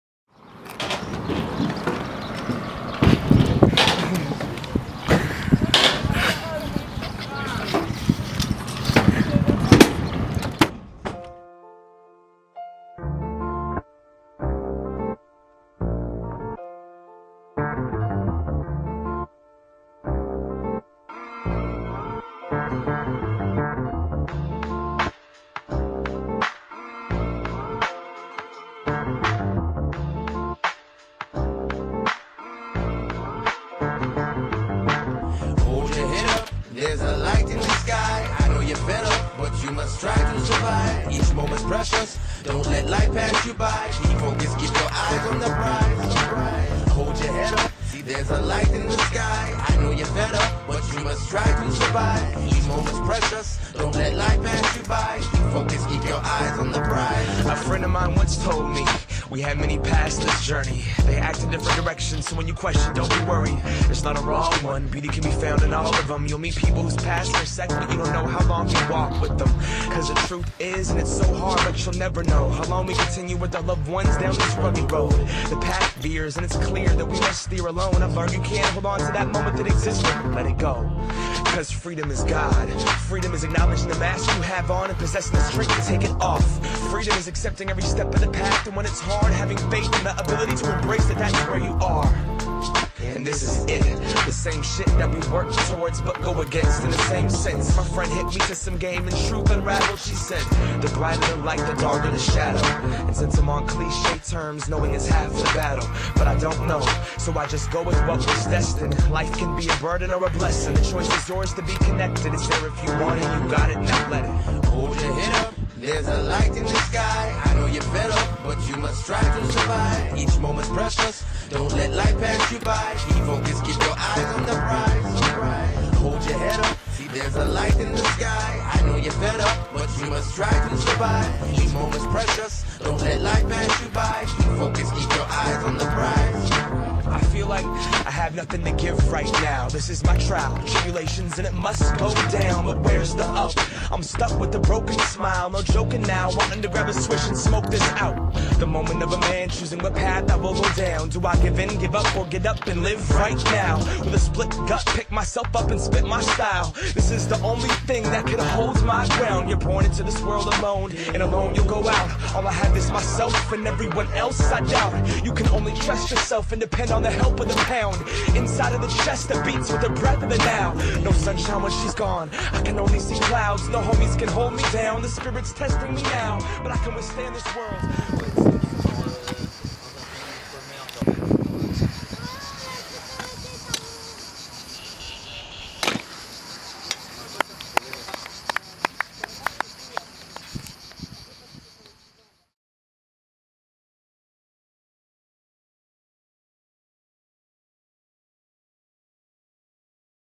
Mathieu Florent | Quick Edit in South 2012
This is the edit Florent Mathieu filmed over a few days during his "down" in the south during the last summer holidays. The clips were filmed in the skateparks of Fréjus, Hyères, La Londe Des Maures, Carnoules, and in the street in the city of Fréjus. Thanks to his father for journeys.

Filming & Editing by Mudry Victor - Canon EOS t2i, Canon 50mm F1.8, Samyang 8mm - Adobe Premiere Pro CS5.